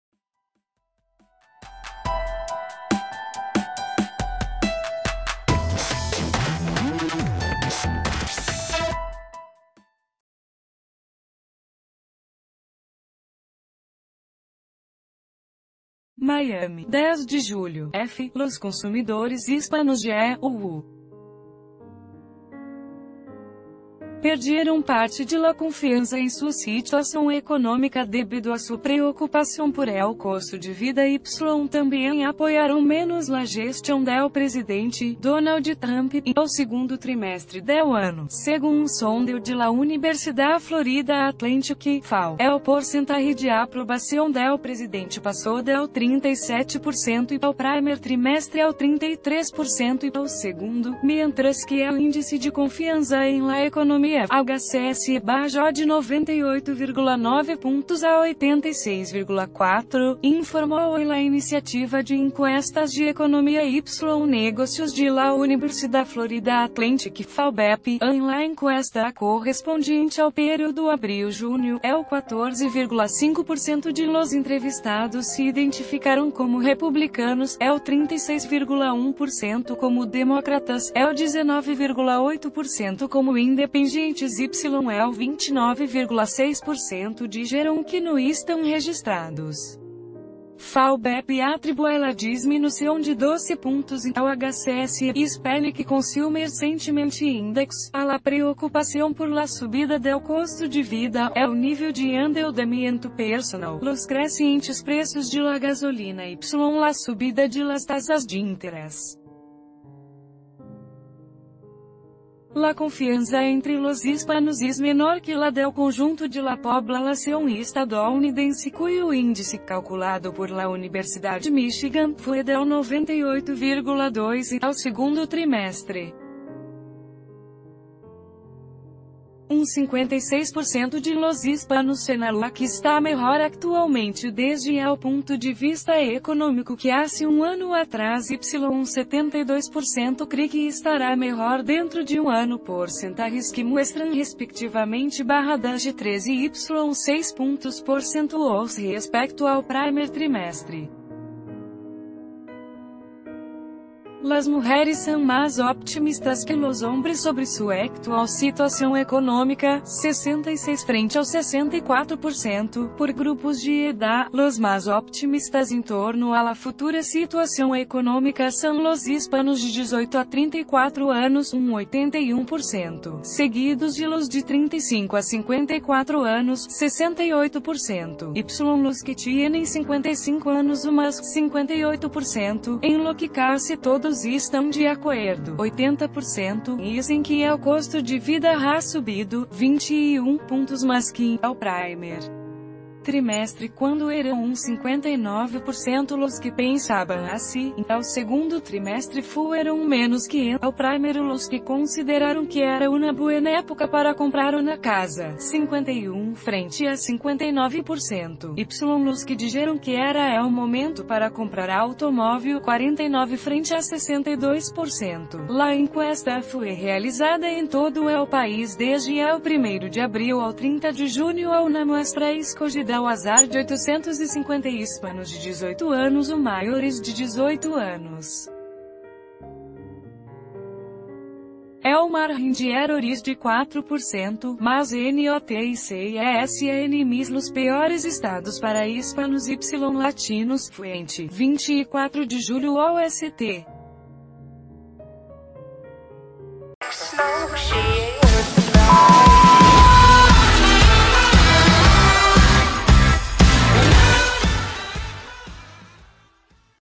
Inquietud por el costo de vida resta confianza al consumidor latino de EE.UU.
Miami, 10 jul (EFE).- Los consumidores hispanos de EE.UU. perdieron parte de la confianza en su situación económica debido a su preocupación por el costo de vida y también apoyaron menos la gestión del presidente, Donald Trump, en el segundo trimestre del año, según un sondeo de la Universidad Flori...